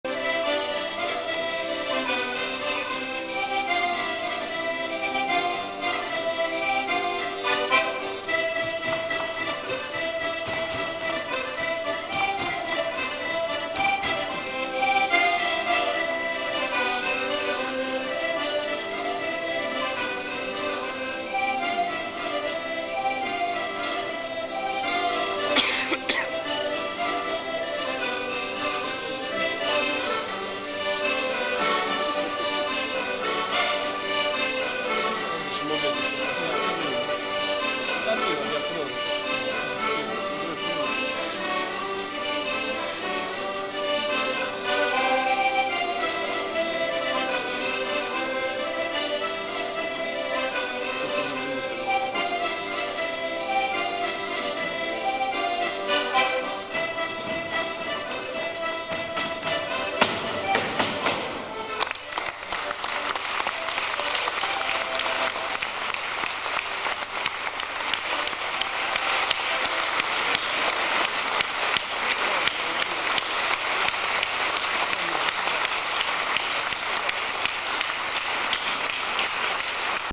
25 4 97 Germania - Esibizione sezione Folk col coro Bachis Sulis.
Il Coro Bachis Sulis, con l'allora Maestro Gianni Garau e una nutrita presenza di accompagnatori tale da poter presentare anche una sezione FOLK, venne invitato per la seconda volta dal 22 al 29 aprile 1997 in Germania accolta ovunque calorosamente - Ulm, Augsberg, Monaco  - . Tutti gli spezzoni filmati relativi a quel viaggio girati in modo amatoriale ed estemporaneo e quindi  non sempre in postazione favorevole sono riversati in digitale dall'archivio personale analogico di Tonio Meloni. Durante il loro soggiorno ogni momento, in visita ai luoghi, alle città o conviviali, oltre quelli ufficiali e programmati, è propizio per esibirsi ora nei canti ora nel ballo ricevendo applausi e segnali di ottimo gradimento dai convenuti o passanti per caso!    Questo è uno di quei momenti.